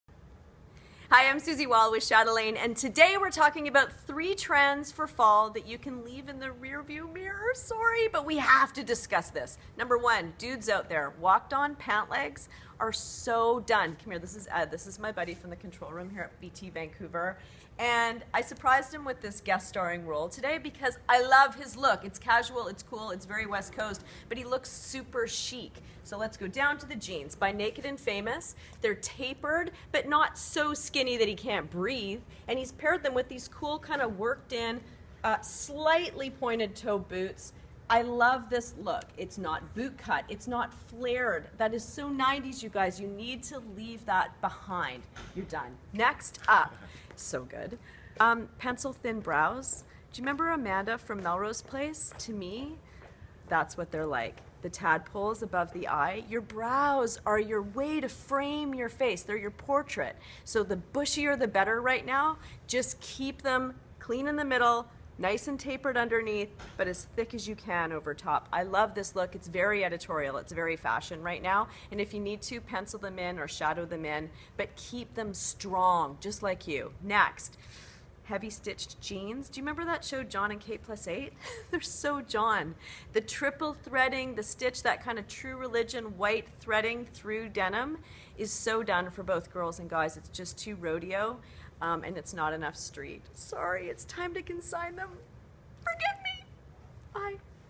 BT Extra 3 Trends It's Time to Pass On
Chatelaine Style Expert Susie Wall identifies three style trends it's time to shelve for good! 2 for women and 1 for the guys.